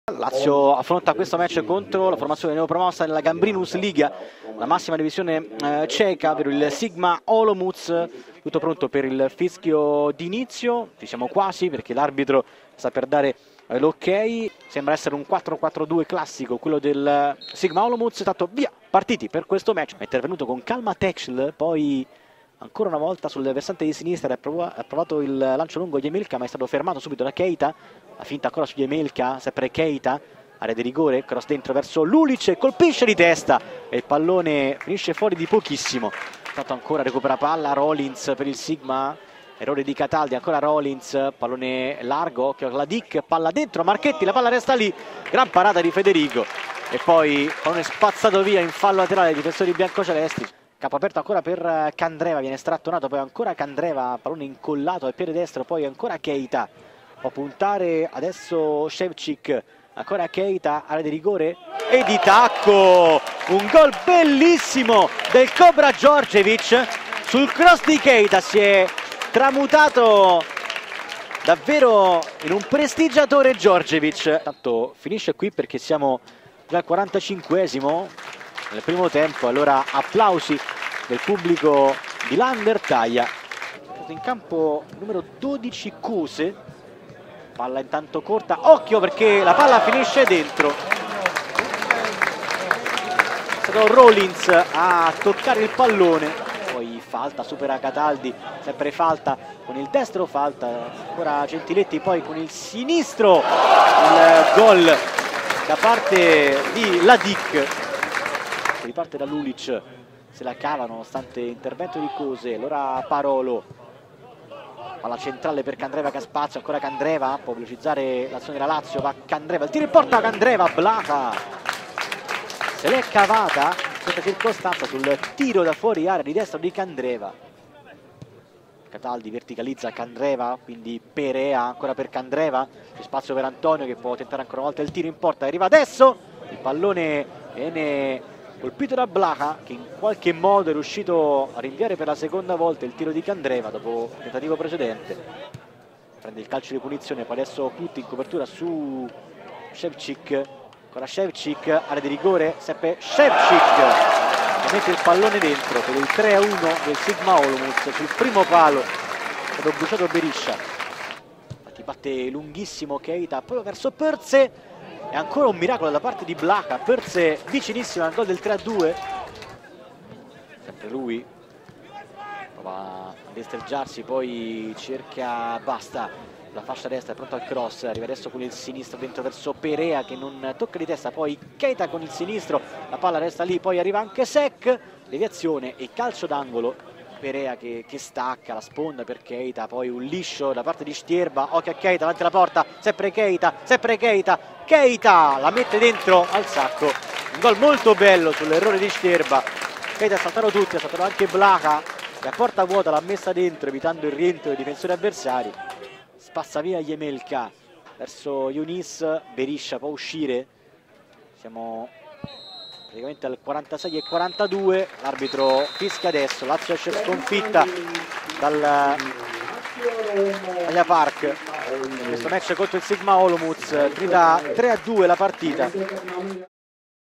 PRE- SEASON 2015, AMICHEVOLE SIGMA OLOMOUC- LAZIO 3-2
Gli highlights tratti da Lazio Style Channel (canale 233 di Sky) dell'amichevole della Lazio contro il Sigma Olomouc andata in scena allo Stadio Laa an der Thaya in terra austriaca il 26 Luglio 2015.